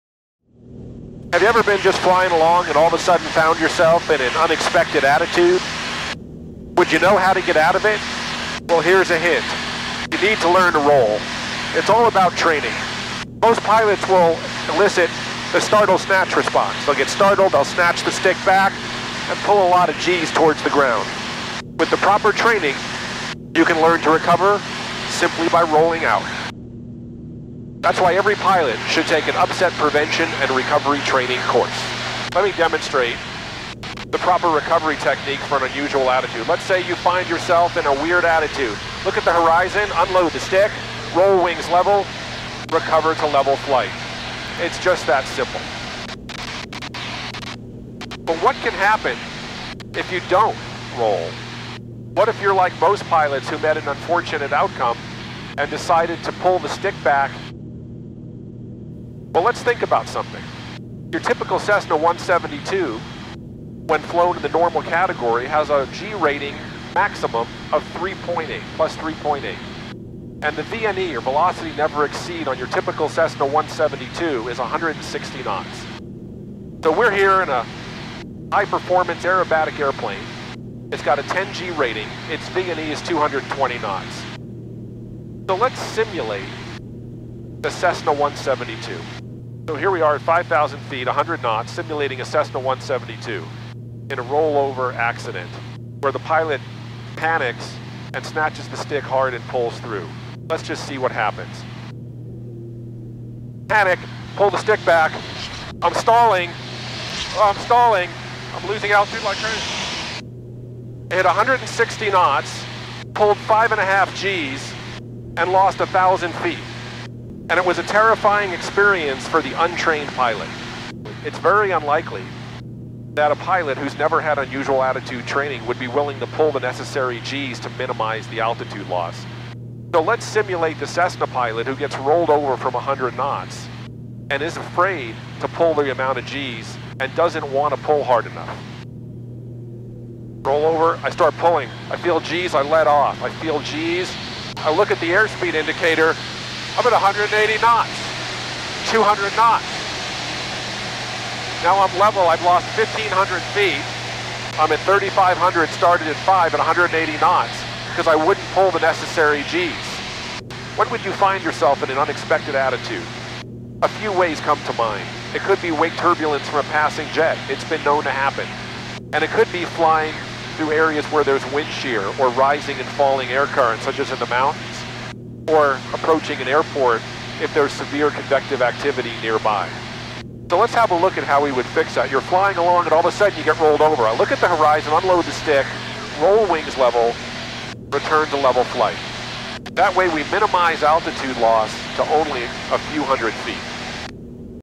Turns, Stalls & Spins - Episode 8: Airplane Recovery From An Unexpected Attitude
In this final video of the series you will see the right way to recover from unexpected airplane upsets and what can go wrong if the pilot is not trained.  No pilot has invented a new way to crash an airplane yet, so learn from others and avoid adding yourself to the accident statistics.

00:00 Introduction
00:32 Inverted flight recovery demonstration
00:51 What if you pull instead of roll?
01:29 Cessna 172 upset simulation with startle-snatch response...SCARY!!
02:18 Cessna 172 upset simulation with overspeed (beyond Vne)
02:40 How unexpected attitudes can happen
03:00 Look - Unload - Roll - Recover

Interested in advancing your airmanship skills, learning aerobatics, or upset prevention and recovery training (UPRT)?

WARNING: Do not perform any maneuver shown in this video unless you have received proper training from a qualified flight instructor in an appropriate aircraft.  Do not perform aerobatic maneuvers in any plane unless it's designed and certified for aerobatics.